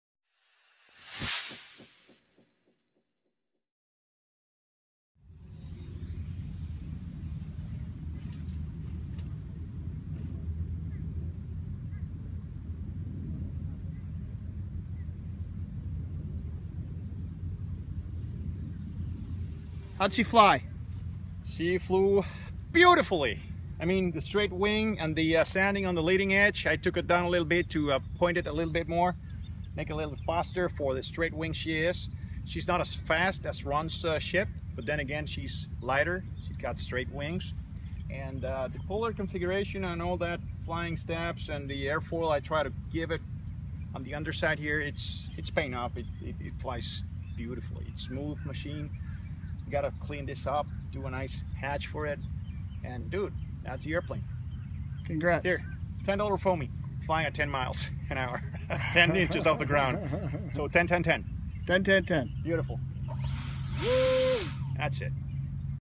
$10 Foamy Converted to RC Straight Wing
A Target purchased $10 foam glider was modified into RC craft using brushless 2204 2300 kV, 12 amp ESC and 6 gram servos and Futaba receiver. Airplane was modified to remove dihedral and swept back wings into a straight wing acrobatic performer. Results are shown and discussed in video.

10x10x10 = 10 cubed (she earned this name)